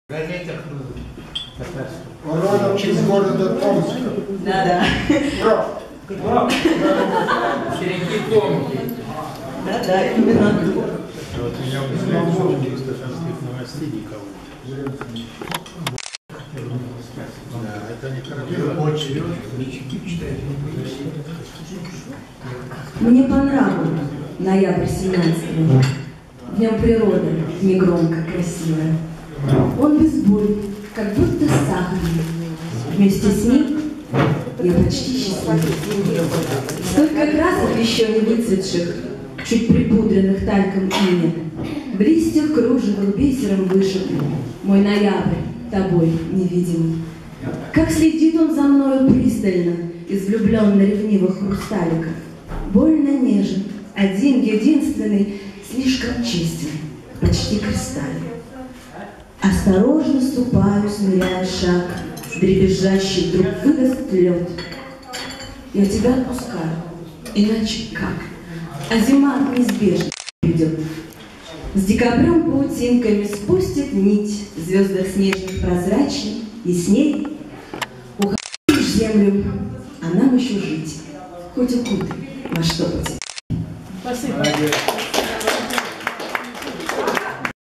19 "Ликуй, моя душа!"
Презентация книги Ахата Мухамедова "Ликуй, моя душа!". Библиотека Татарского культурного центра.